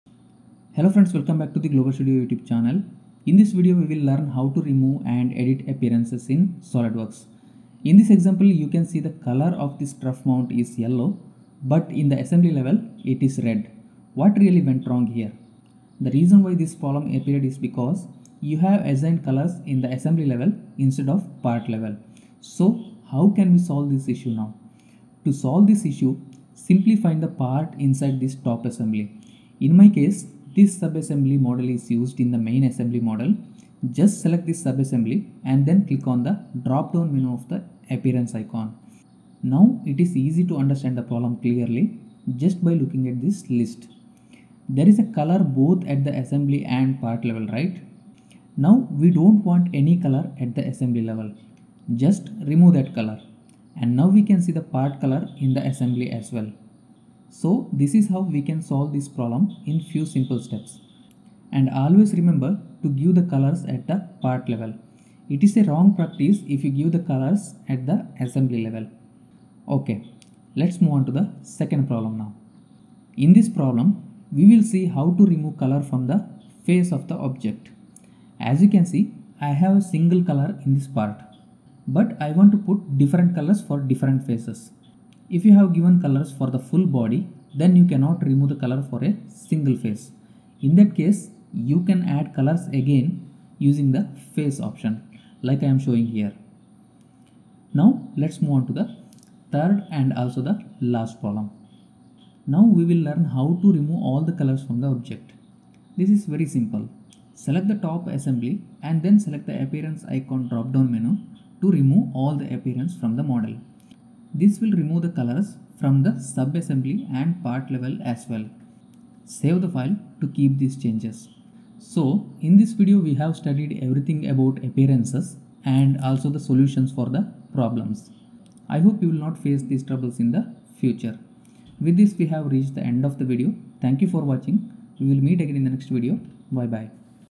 HOW TO REMOVE AND EDIT APPEARANCES IN SOLIDWORKS? SOLIDWORKS TUTORIAL
In this SolidWorks tutorial, we will cover how to easily remove and edit appearances in your designs. Watch as I walk you through the step-by-step process of updating the appearance of your 3D models to achieve the desired look.

Whether you are a beginner or experienced SolidWorks user, this video will provide you with valuable tips and tricks to enhance the visual appeal of your designs. Don't miss out on this must-watch tutorial!